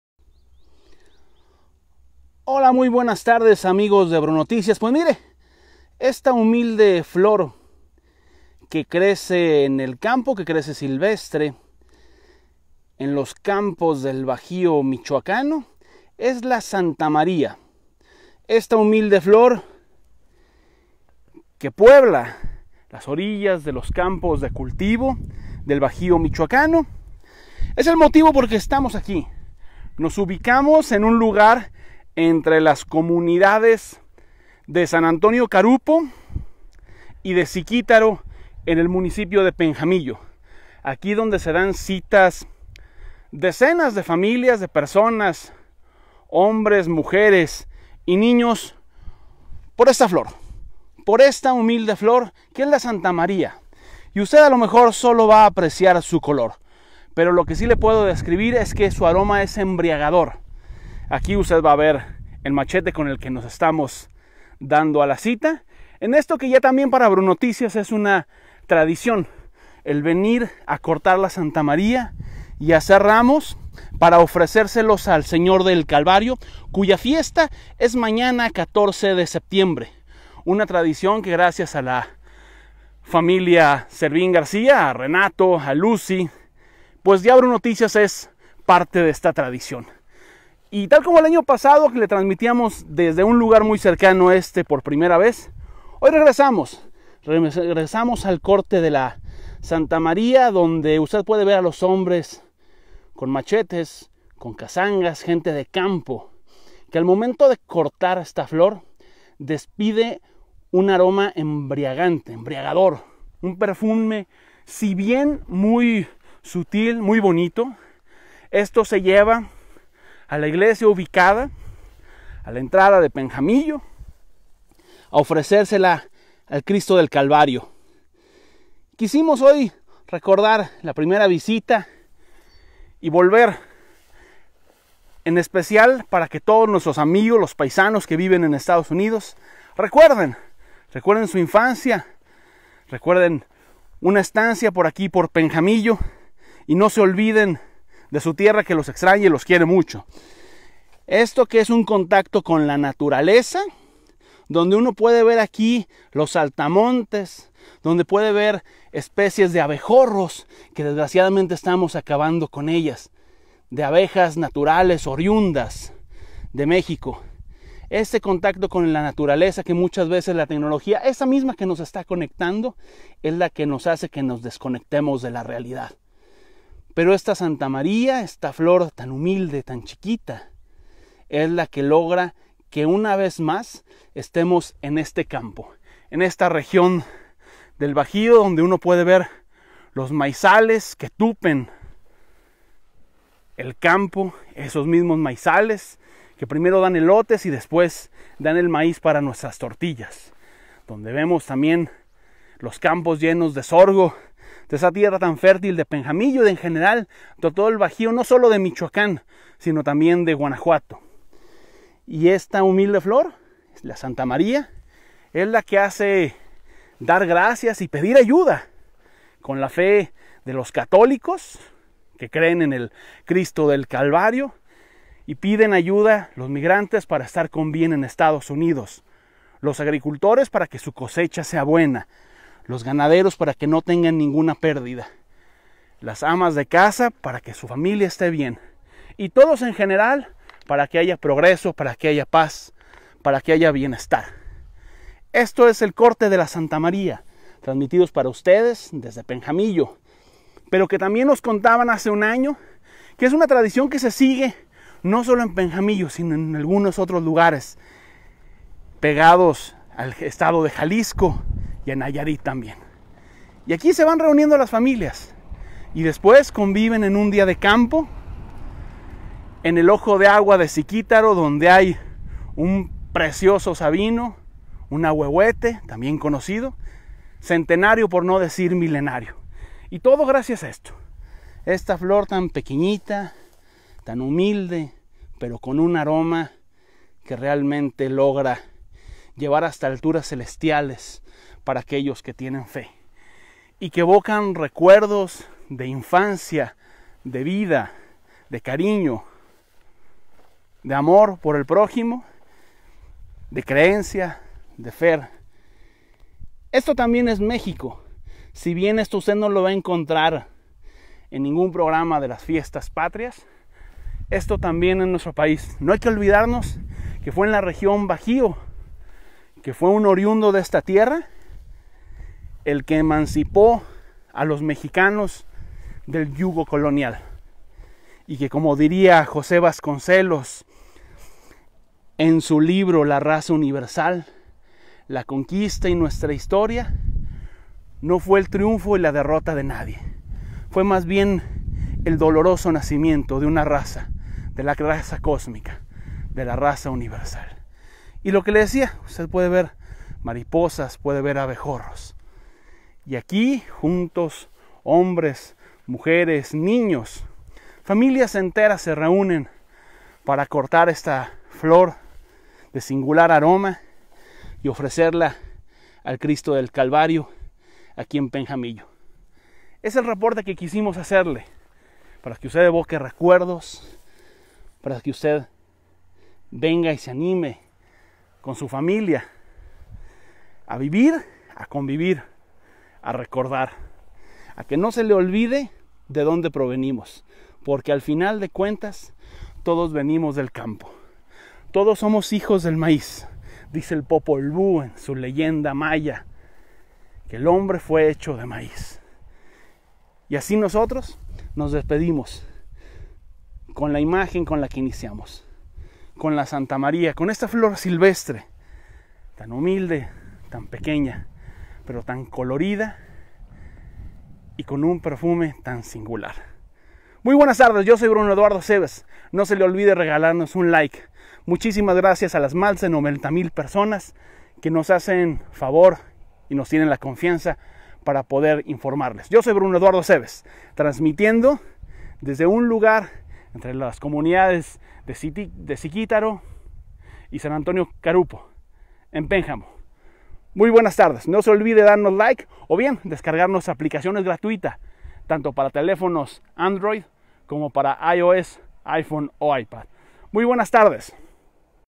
RECOLECCIÓN DE LA "SANTA MARÍA" EN PENJAMILLO
EL 14 DE SEPTIEMBRE SE FESTEJA EN PENJAMILLO MICHOACÁN AL CRISTO DE EL CALVARIO. FAMILIAS ENTERAS ACUDEN AL CAMPO PARA RECOLECTAR UNA FLOR SILVESTRE LLAMADA "SANTA MARÍA" MISMA QUE DESPIDE UN PERFUME MUY SINGULAR Y LA OFRECEN EN EL TEMPLO.